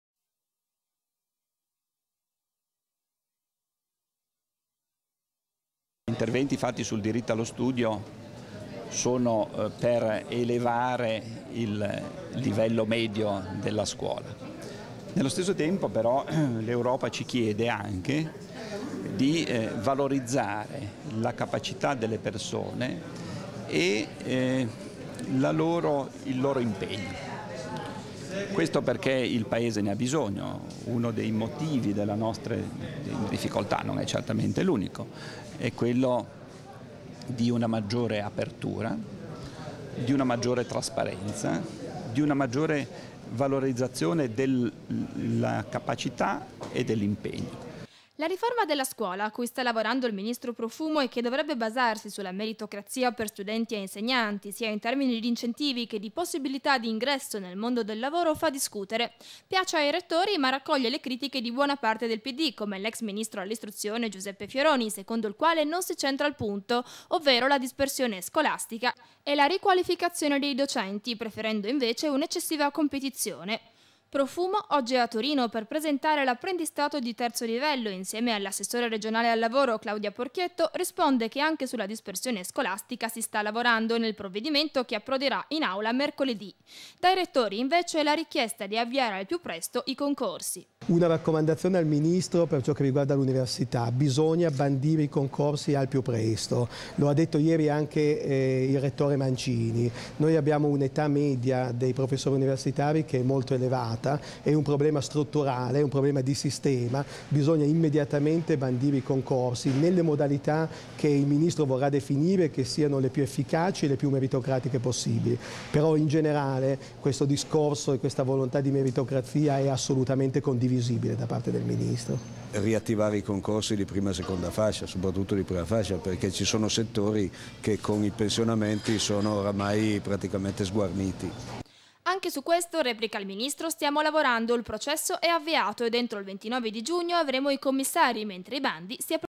Profumo in visita a Torino
Giornata torinese oggi anche per il Ministro Francesco Profumo che ha presentato insieme all'assessore regionale Claudia Porchietto, l'apprendistato di terzo livello alla presenza dei rettori. A tenere banco però è la riforma dell'istruzione  che verrà presentata mercoledì in aula.